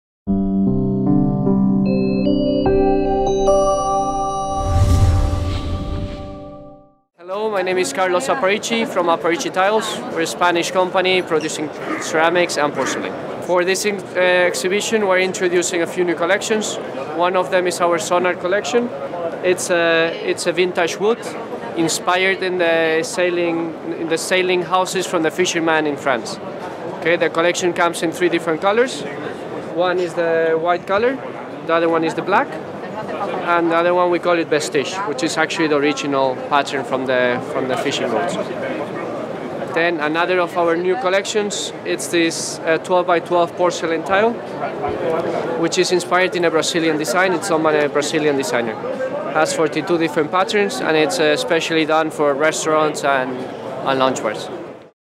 Carlos Aparici of Aparici Tiles at Coverings 2013
Carlos Aparici of Aparici Tiles talks about some of their new collections they're showcasing at Coverings 2013 in Atlanta.